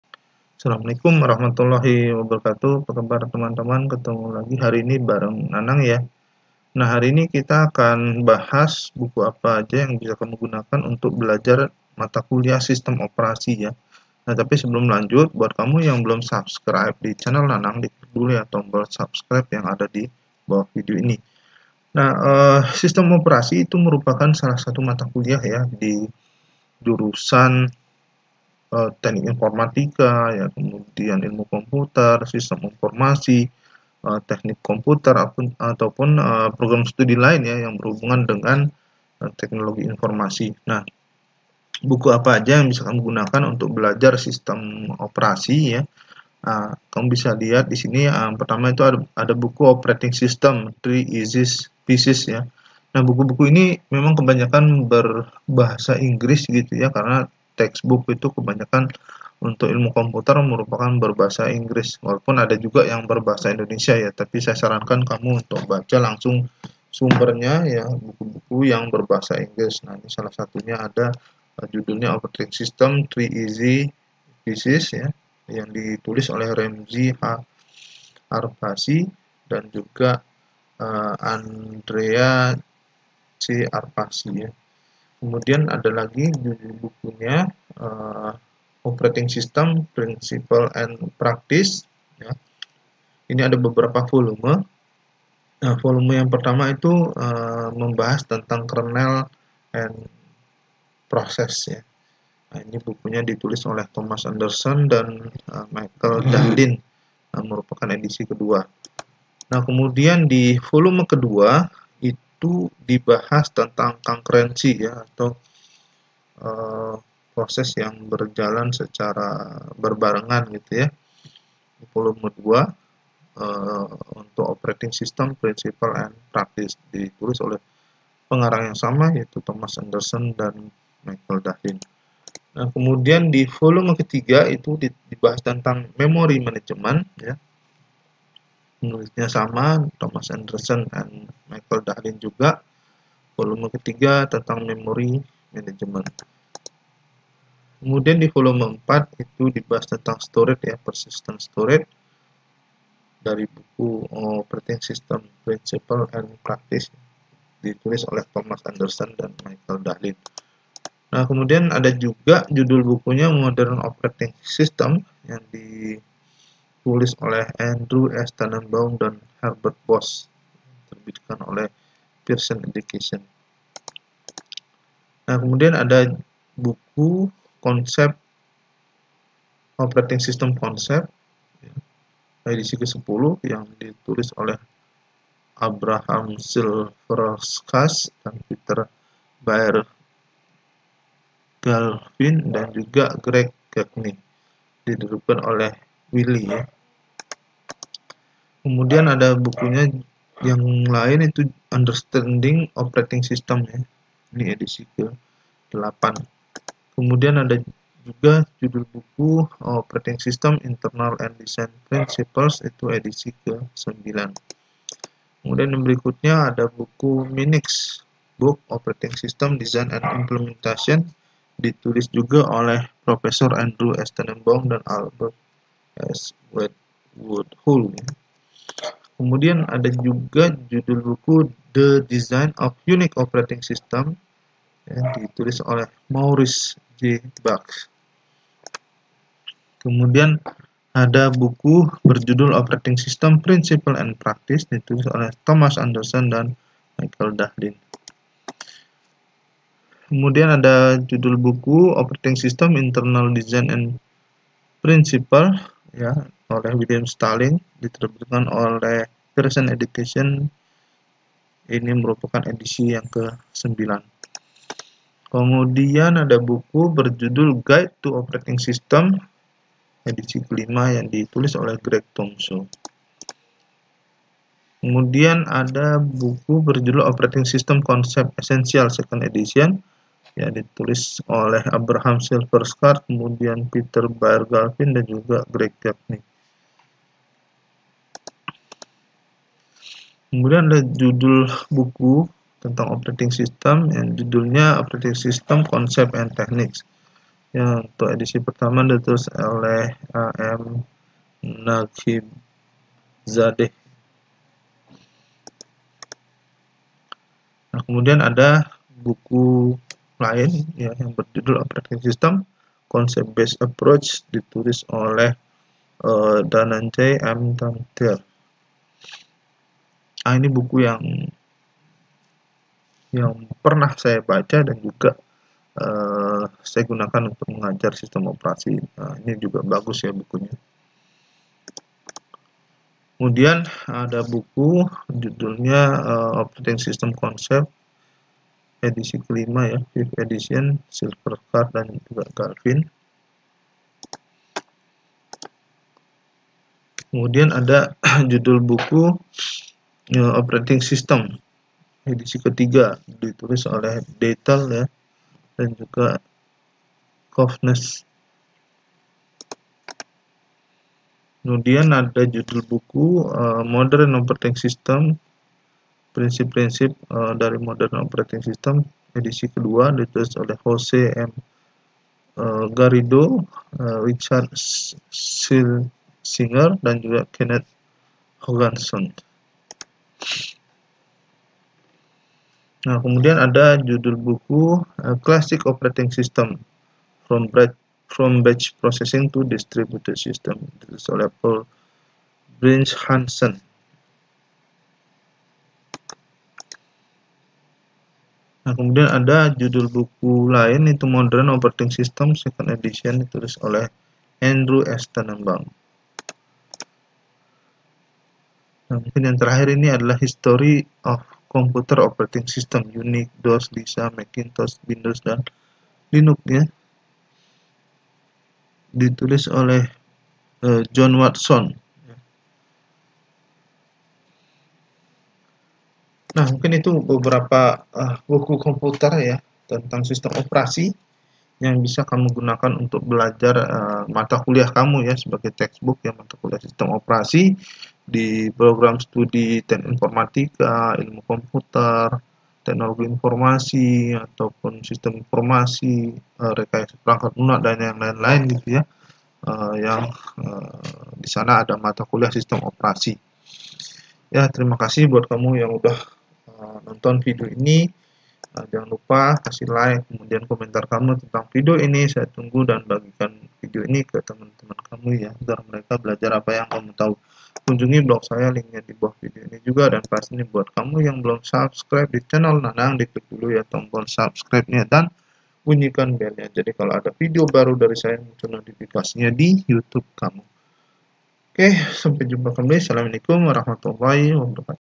Buku Belajar Sistem Operasi Operating System Books
Di dalam video ini saya akan menjelaskan tentang buku untuk belajar mata kuliah sistem operasi.

Untuk artikel dan tutorial seputar IT kamu bisa kunjungi web saya berikut ini:

Untuk artikel dan tutorial seputar Reading, Travelling dan karir kamu bisa kunjungi web saya berikut ini: